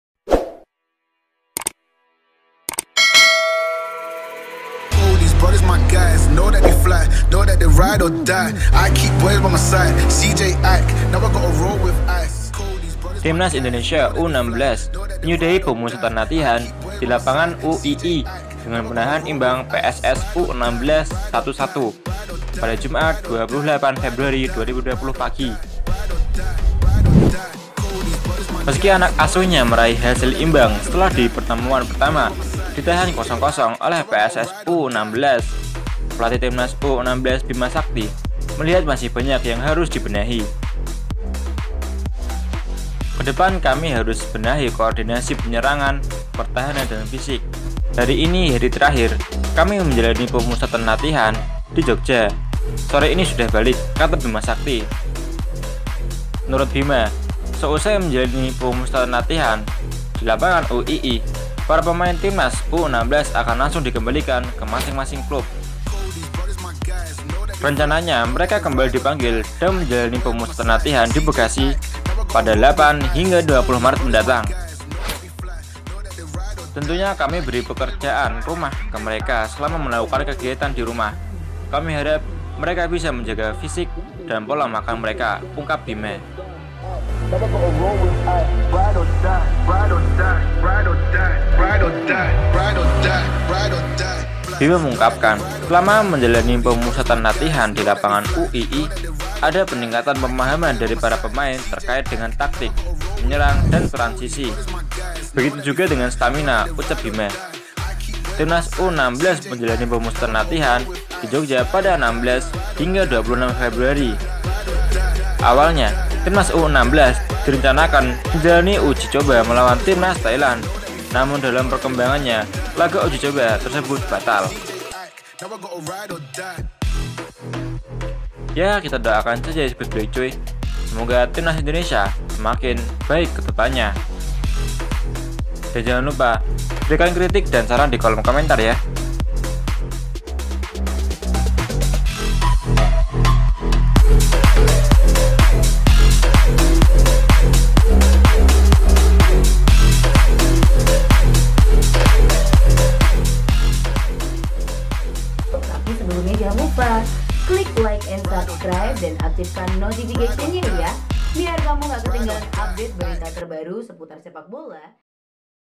ADU BANTENG🔥HASIL LAGA UJICOBA TIMNAS U-16 TADI PAGI DI YOGYAKARTA
Timnas Indonesia U-16 menyudahi pemusatan latihan di Lapangan UII, dengan menahan imbang PSS U-16 1-1, Jumat (28/2/2020) pagi.
Meski anak asuhnya meraih hasil imbang setelah di pertemuan pertama ditahan 0-0 oleh PSS U-16, pelatih Timnas U-16 Bima Sakti melihat masih banyak yang harus dibenahi.
“Ke depan kami harus benahi koordinasi penyerangan, pertahanan dan fisik. Hari ini hari terakhir kami menjalani pemusatan di Jogja, sore ini sudah balik,” kata Bima Sakti.
Menurut Bima, seusai menjalani pemusatan latihan di Lapangan UII, para pemain Timnas U-16 akan langsung dikembalikan ke masing-masing klub. Rencananya, mereka kembali dipanggil dan menjalani pemusatan latihan di Bekasi, 8-20 Maret mendatang.
“Tentunya kami beri pekerjaan rumah ke mereka selama melakukan kegiatan di rumah. Kami harap mereka bisa menjaga fisik dan pola makan mereka,” ungkap Bima.
Bima mengungkapkan, selama menjalani pemusatan latihan di Lapangan UII, ada peningkatan pemahaman dari para pemain terkait dengan taktik, menyerang dan transisi. “Begitu juga dengan stamina,” ucap Bima.
Timnas U-16 menjalani pemusatan latihan di Jogja pada 16 hingga 26 Februari. Awalnya, Timnas U-16 direncanakan menjalani laga uji coba melawan Timnas Thailand, namun dalam perkembangannya, laga uji coba tersebut batal.